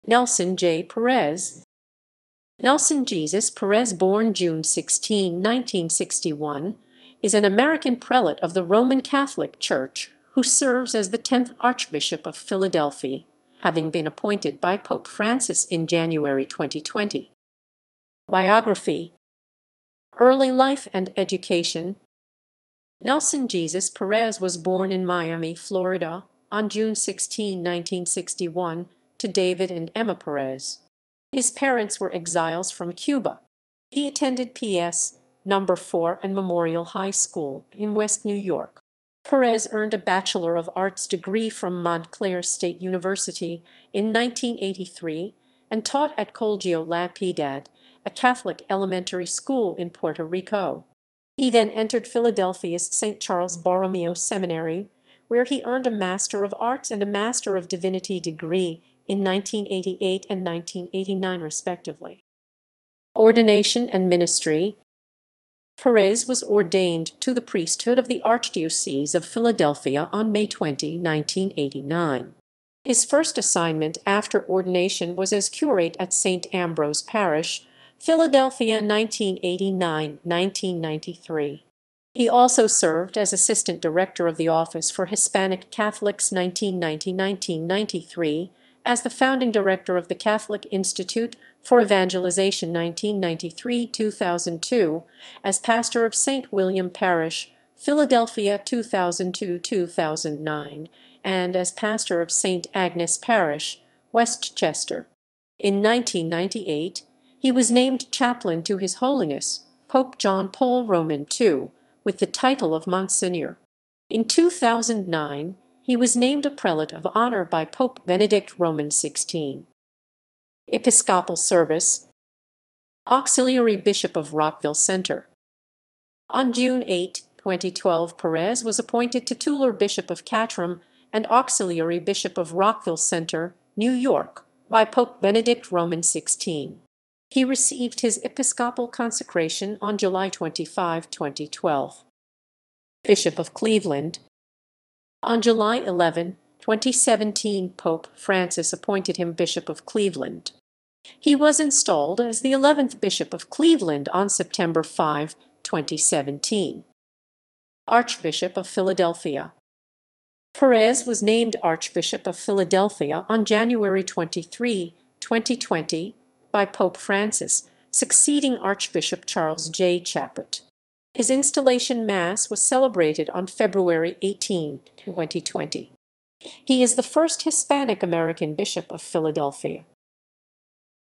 Nelson J. Perez
Nelson Jesus Perez (born June 16, 1961) is an American prelate of the Roman Catholic Church who serves as the 10th Archbishop of Philadelphia, having been appointed by Pope Francis in January 2020.